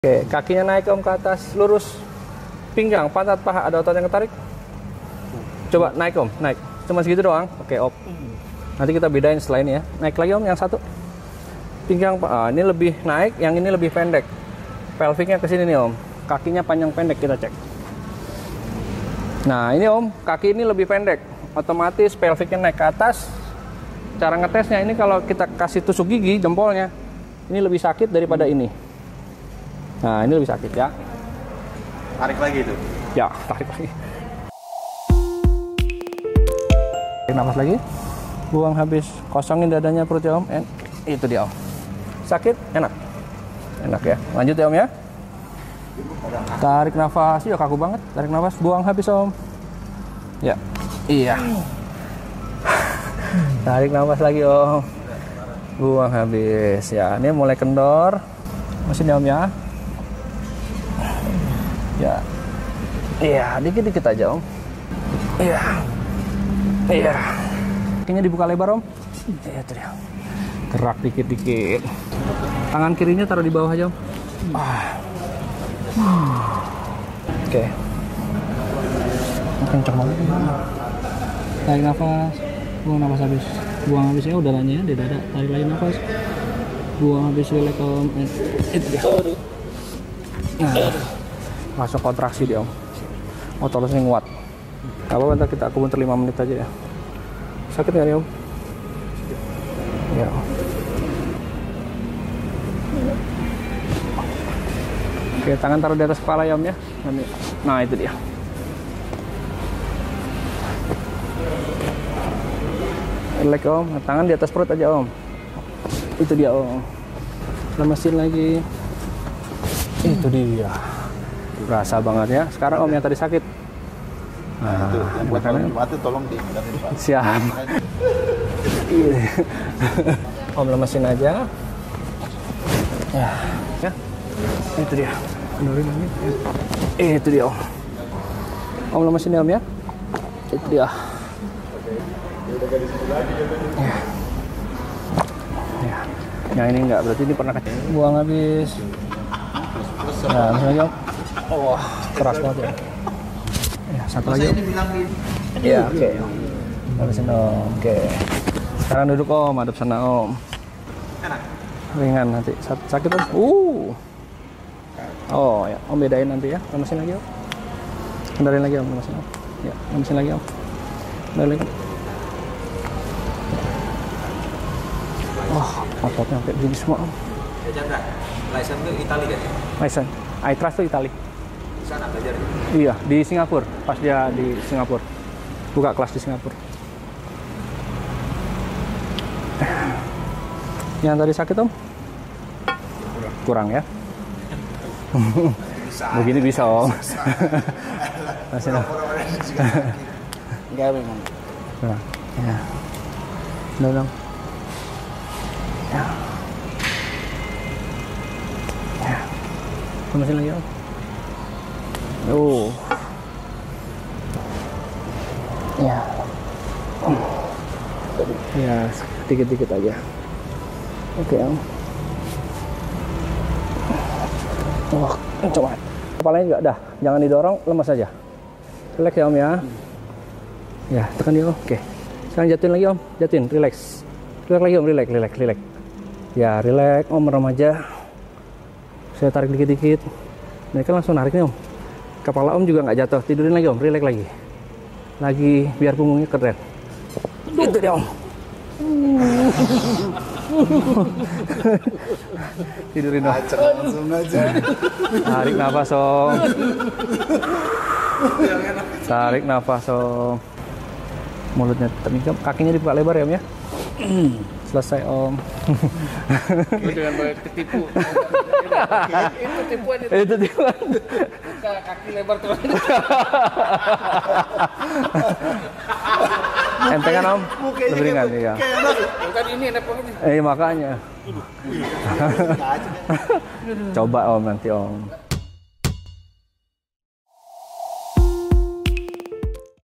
OM SUHERLI, BOS HOG ANAK ELANG COBAIN BONTE, ASLI INI BUKAN ABAL ABAL, BIKIN BADAN ENAK BANGET.
Sosialmedia @bonte.center
IG & Tiktok @bonte.center @chandrawahyuhs